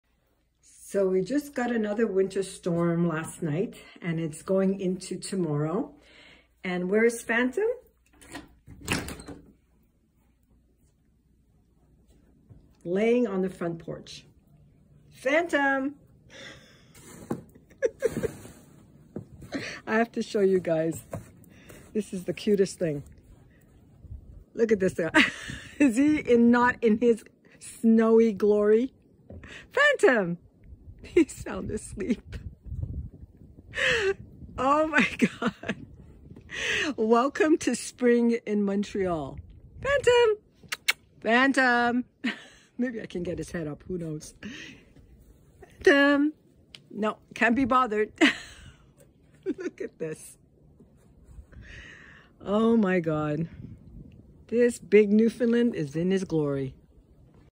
Mom go away I’m not DEAD I’m sleeping. Phantom Has a doggy door he decides in or out ❄️❤️🇨🇦
Phantom has his own doggy door, so he decides if he’s in or out. Newfoundland dog owners know , they love the cold, so they prefer to be outdoors￼. They will Definitely come in if they’re cold, but that rarely happens❄️😉❤️🇨🇦￼